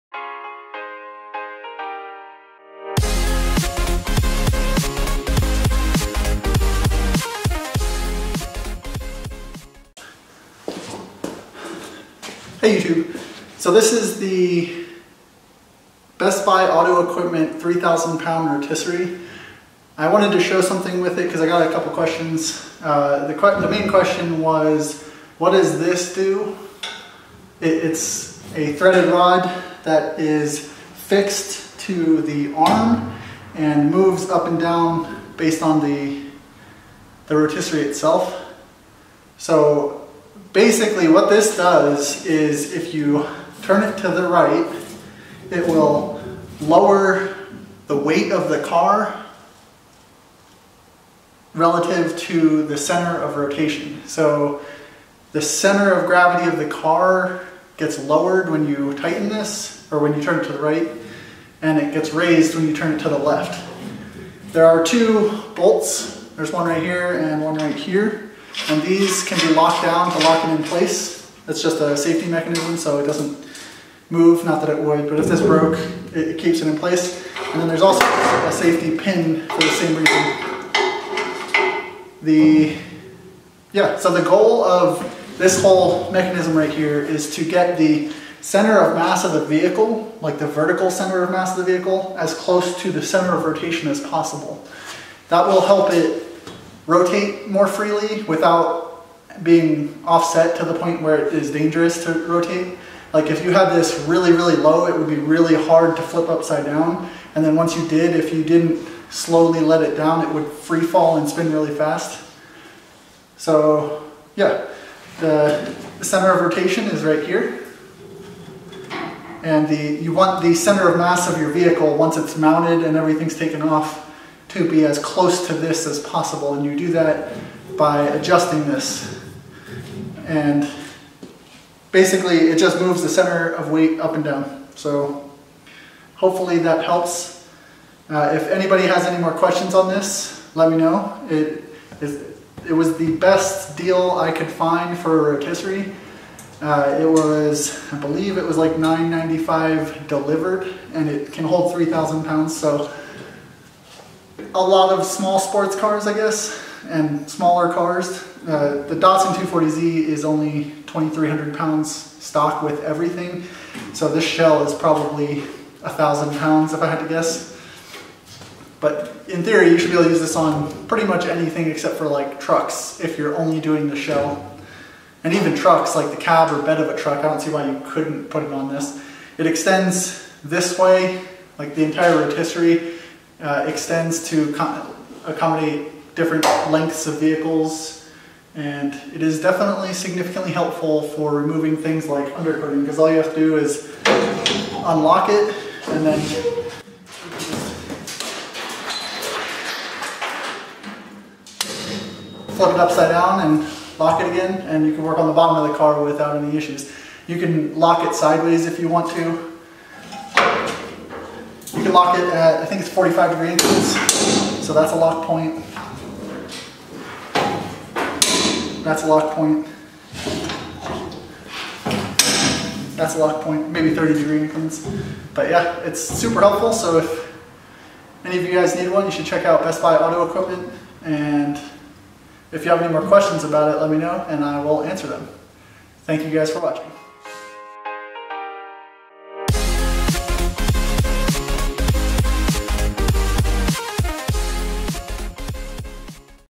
Car Rotisserie Auto Lift BestBuyAutoEquipment CR-3000
In our new series, Brian answers some commonly asked questions about his Automotive Rotisserie.

See how it works and why you should buy one!